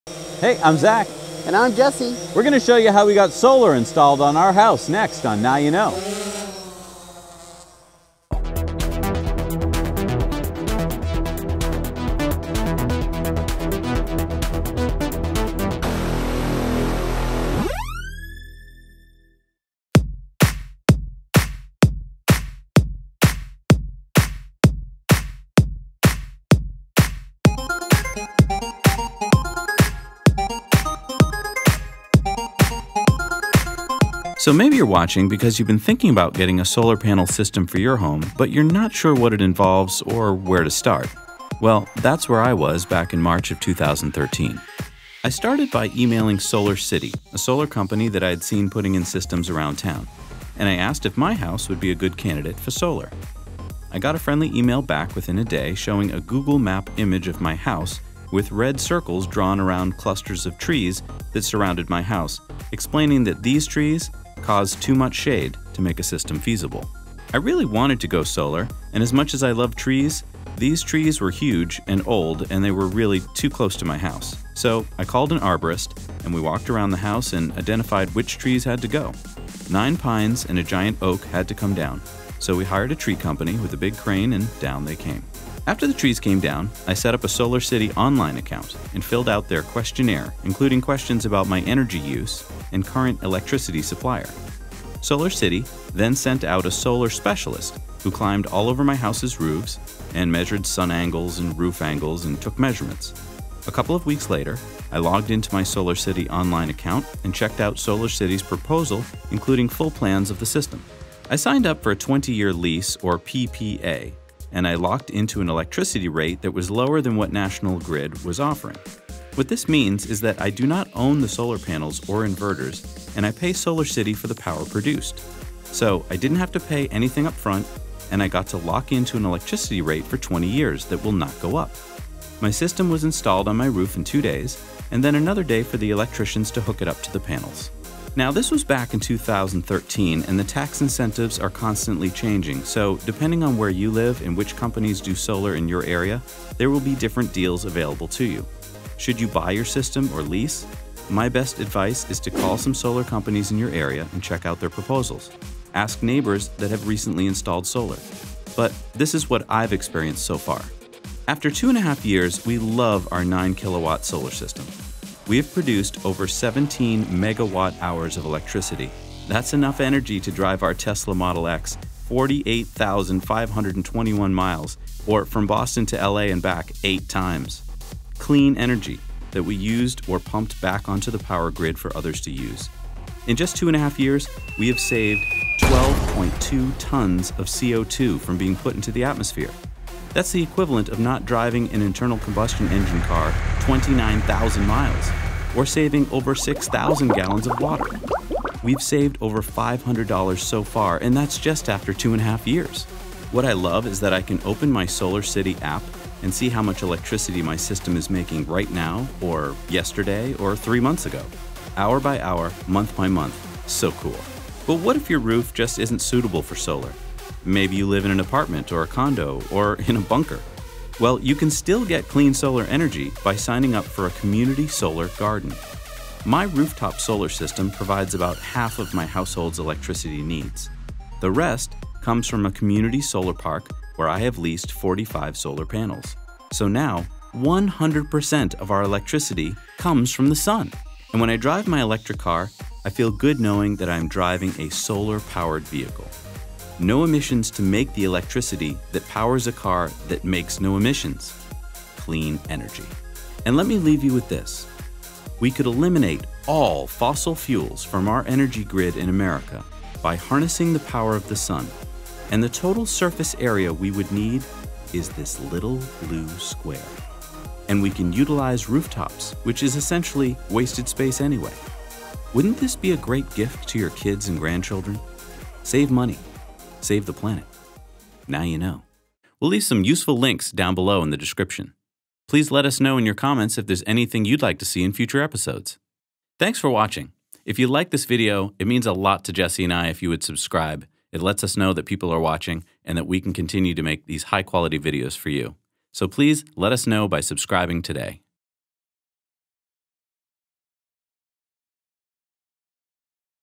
My Solar Installation Story
We share how we got solar panels installed on our house in 2013 and what we've experienced so far.